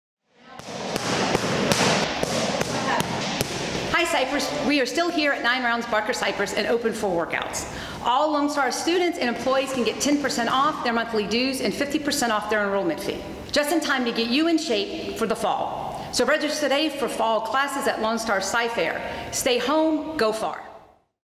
LSC Students and Employees Get Discount at 9ROUND
All LSC students and employees get 10% off their monthly dues and 50% off their enrollment fee at 9ROUND Barker Cypress - just in time for to get you in shape for the fall! Register today for fall classes at Lone Star College-CyFair!